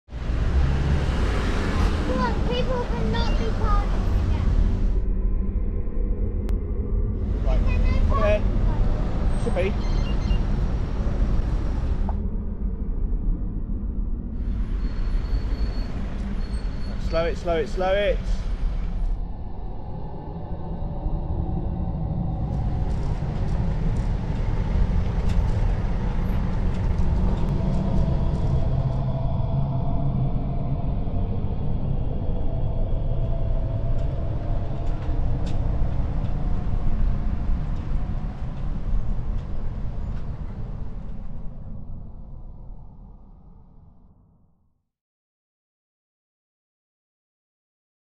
The School Run - Want to take a deep breath?
Exhaust is toxic. So is brake, tyre, and road dust. Do you want to take a deep breath? Driving seriously harms you and others around you.